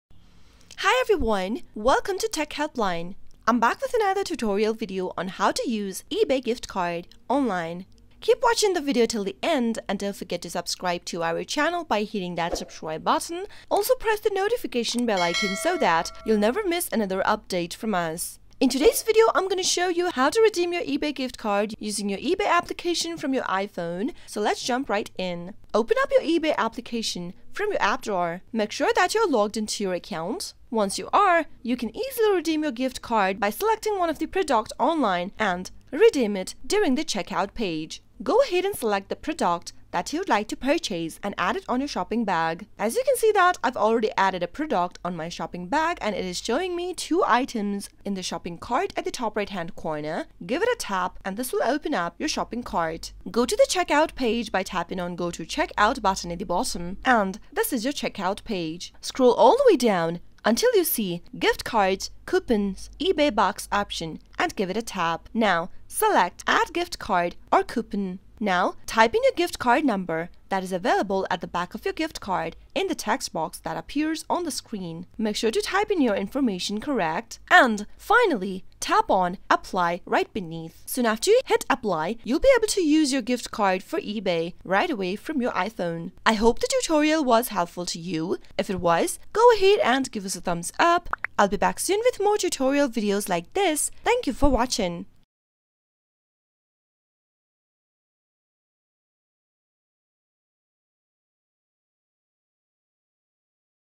How To Use eBay Gift Cards | Redeem an eBay Gift Card Online 2022
In this video I'll show you How To Use eBay Gift Cards.

What could be more perfect than a way to spend your hard-earned money? The eBay gift card. Use it to buy the things you love and get the things you need.

0:00 - Intro
0:18 - How To Use eBay Gift Cards
1:26 - Outro

Hey EveryOne!